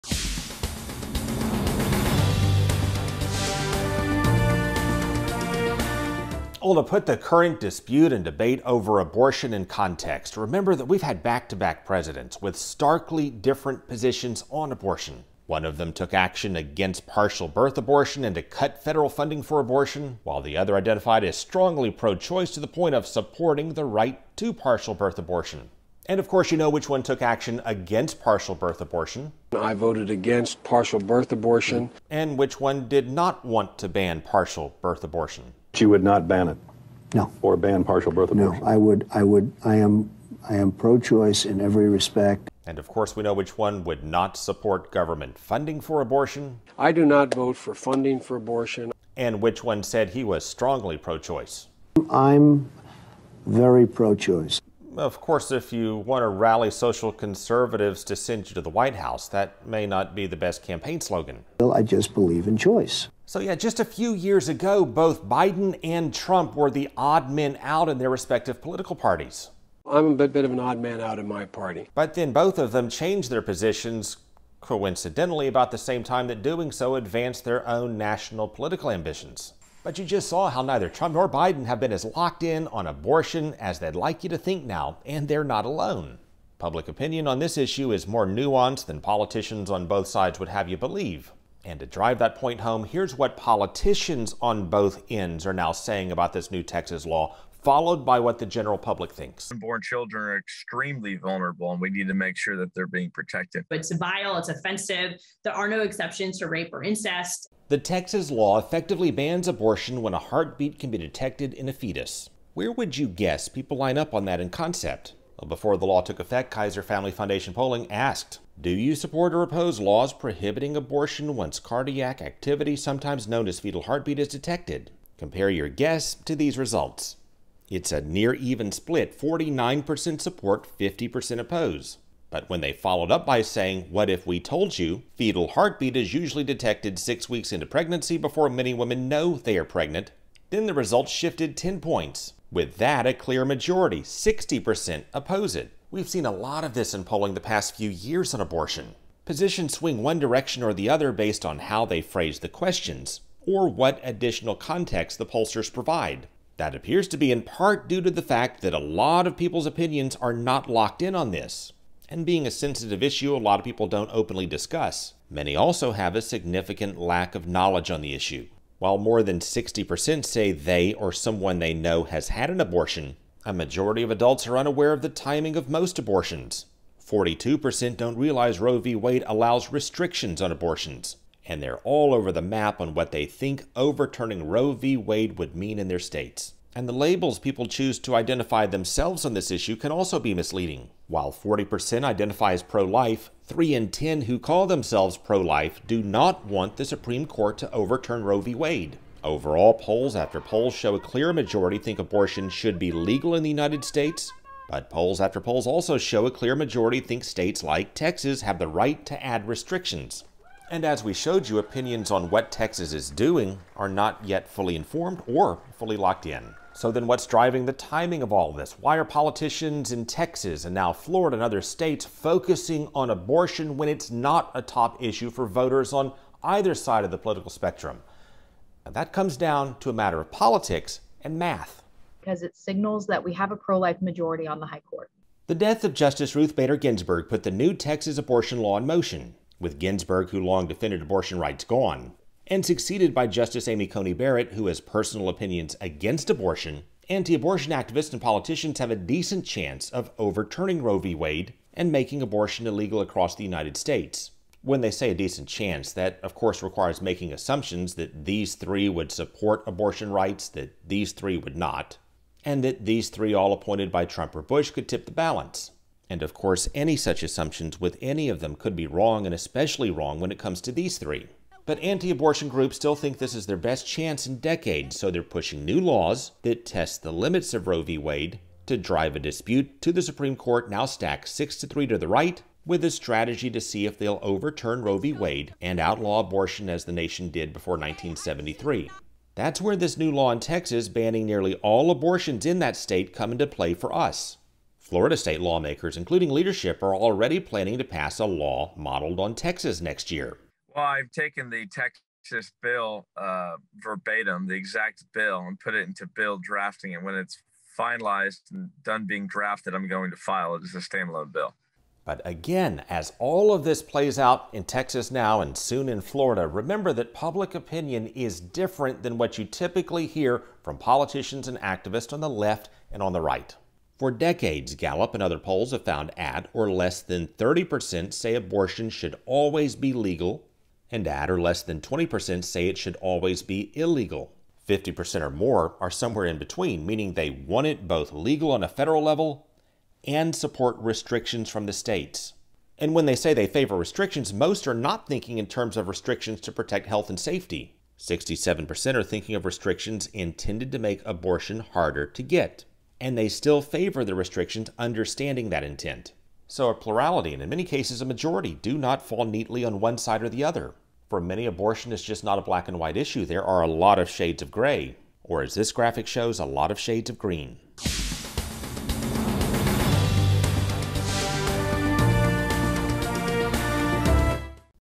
Context on the debate over abortion
Context on the debate over abortion restrictions- and a closer look at public opinion, and the positions of leading politicians that have changed through the years...